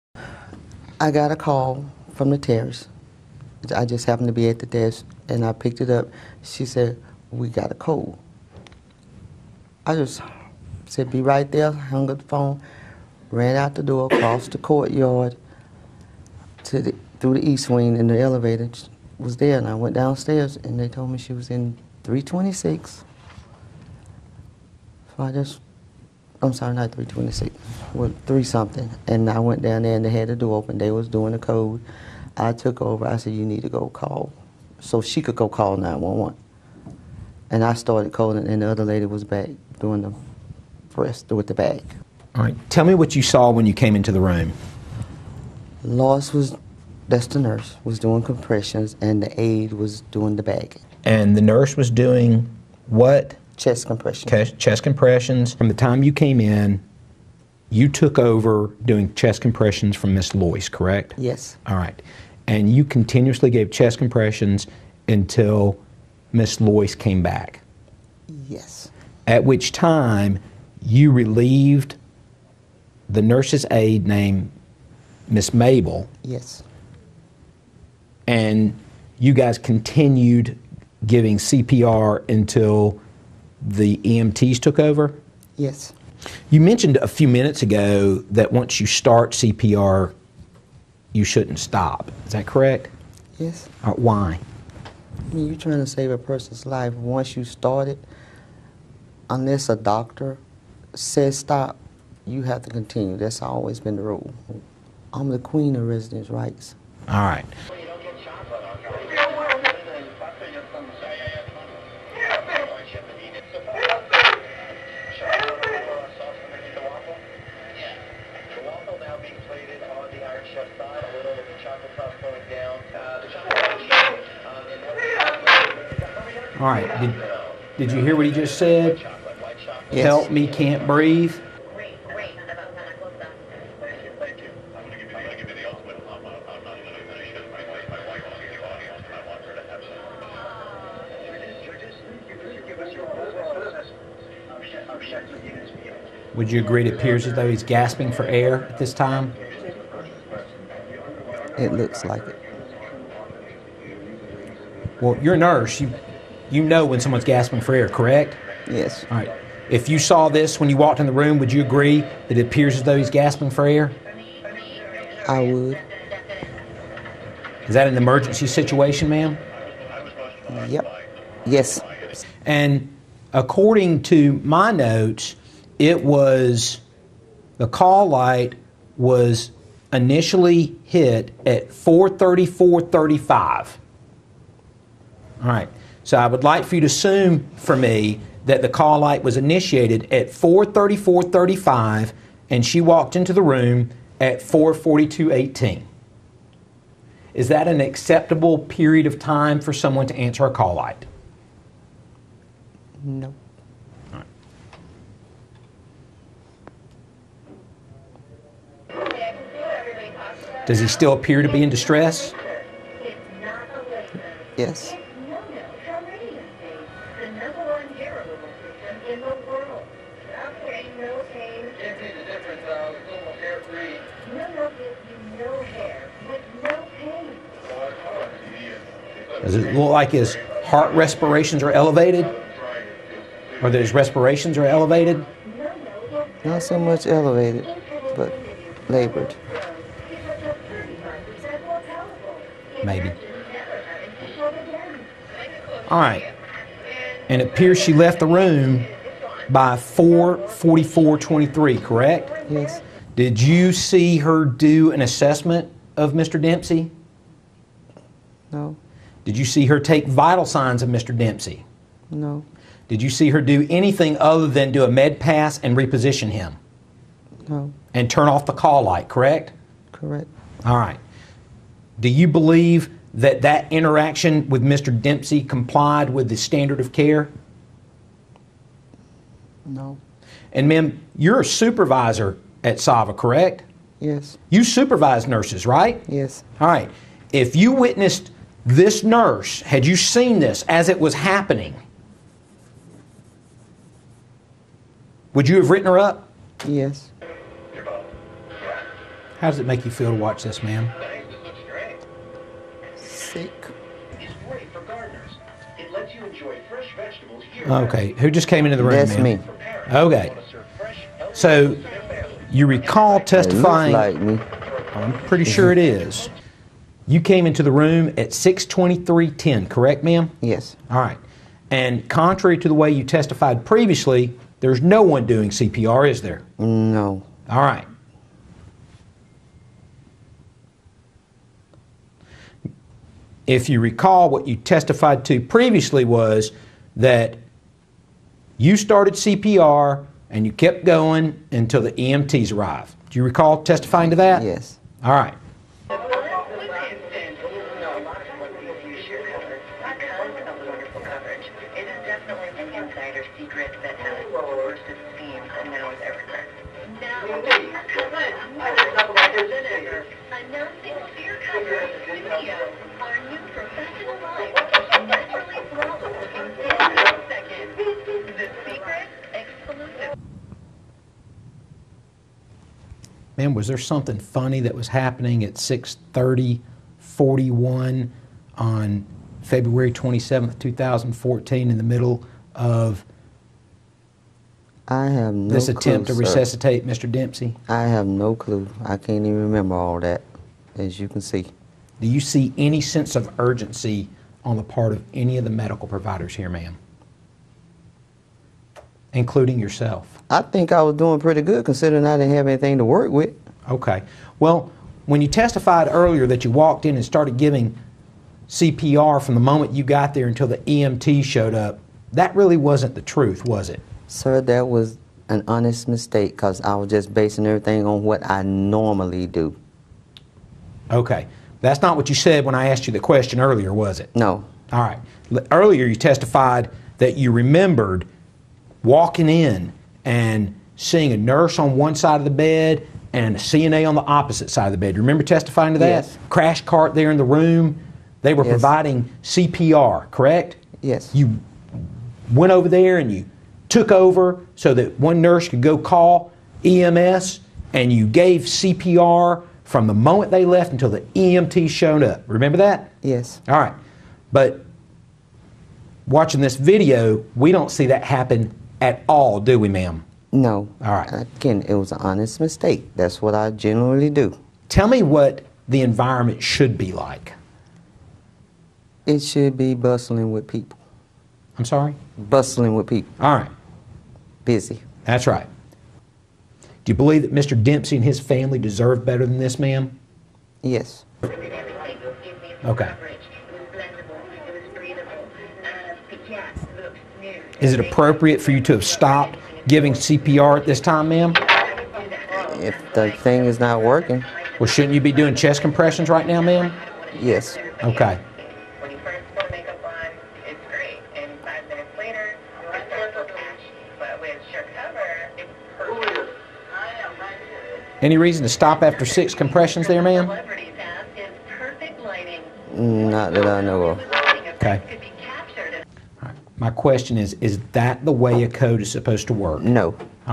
Black Nurse Wanda Nuckles Changes Story on James Dempsey's Death
A black nursing supervisor, Wanda Nuckles who laughed with her fellow black nurses as they deprived essential care from a white, decorated World War 2 veteran,  James Dempsey as he begged for air, gasping for 'help' and eventually died changes her story in her deposition when she watches the hidden camera footage for the first time of her actions.